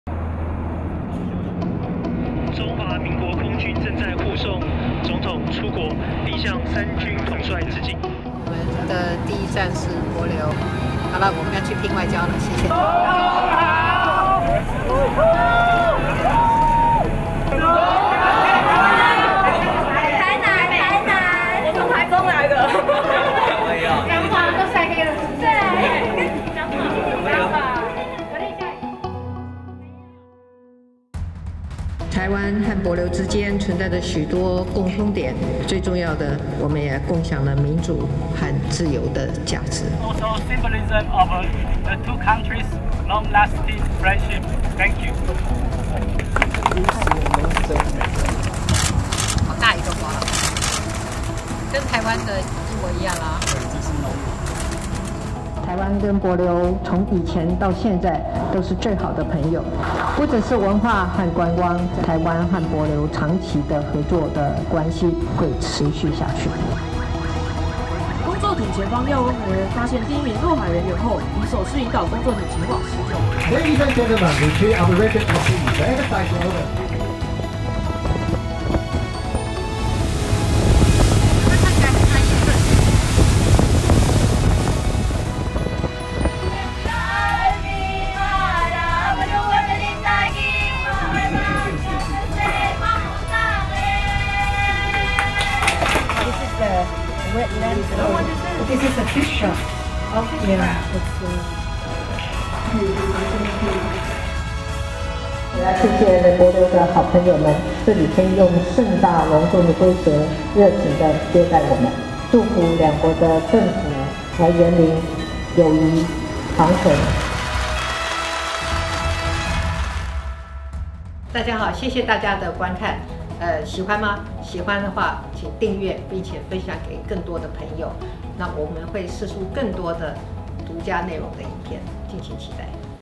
【 總統做什麼 EP2 】跟著小英總統拚外交！海洋民主之旅全紀錄（上集）
小英總統拚外交，邦交國對台灣的幫助真的很大！因為有著互相承認主權的正式外交身分，讓他們在國際場合上可以用力地聲援我們！支持台灣參與國際社會。

小英總統這次出訪太平洋友邦，其實是身負重要的外交任務，除了和友邦領袖、各國駐地大使們會面，把握每一個讓世界看見台灣的機會外，更要關心台灣在各友邦的外交人員和技術團成員，慰勞大家在外交第一線的辛勞。

小英的第一站：帛琉，就是台灣在太平洋的堅定友邦。隨著台灣和帛琉間交流日益密切，台灣直飛帛琉的航班也即將加開，大家趕快換上花襯衫，享受這美麗的海洋民主之旅囉！

想看更多小英總統的影片嗎？